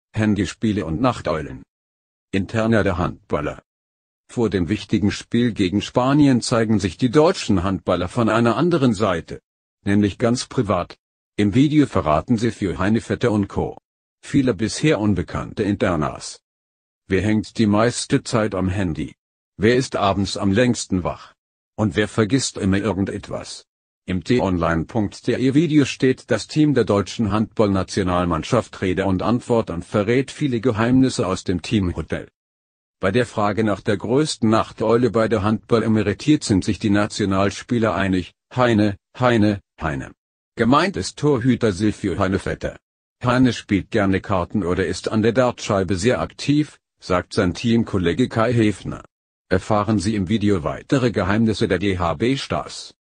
Handball-EM: Die Geheimnisse der deutschen Handballstars
Handyspiele und Nachteulen
Interna der Handballer.
Vor dem wichtigen Spiel gegen Spanien zeigen sich die deutschen Handballer von einer anderen Seite. Nämlich ganz privat. Im Video verraten Silvio Heinevetter & Co. viele bisher unbekannte Internas.
Wer hängt die meiste Zeit am Handy? Wer ist abends am längsten wach? Und wer vergisst immer irgendetwas? Im t-online.